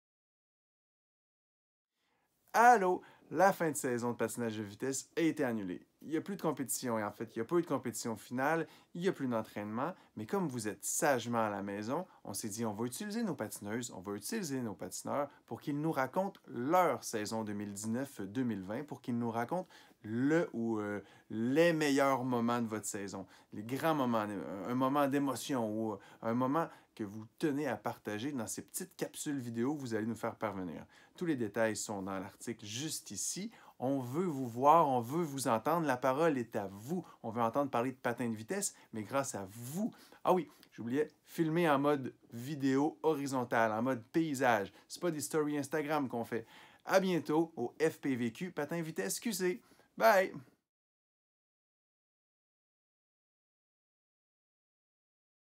Défi: le PatinVitesseQc de chez toi
Comment participer? Tous les détails
Athlètes et coachs, soyez créatifs! La saison s'est terminée de façon beaucoup trop abrupte. Profitez de votre isolement pour partager de bons moments, des anecdotes, des souvenirs de la saison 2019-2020. Les meilleures vidéos seront publiées sur notre chaîne YouTube!